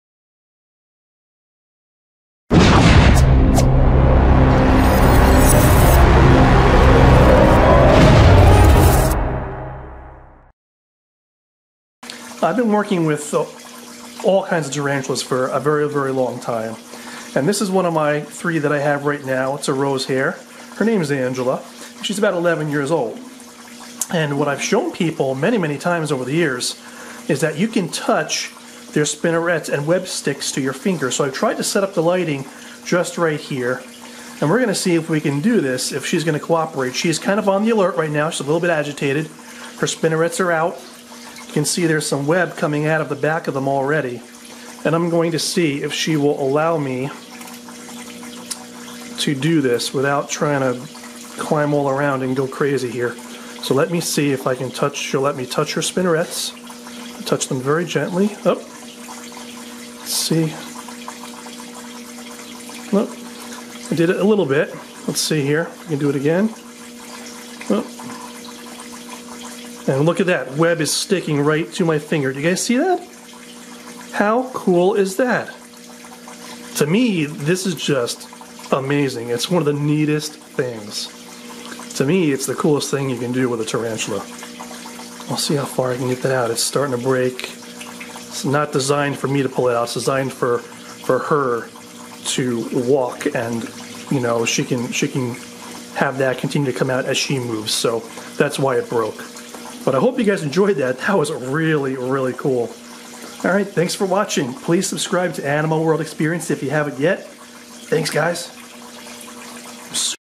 Pulling Web From a Tarantula's Spinnerets
Check out this cool video showing how you can pull web out from a tarantula's spinnerets!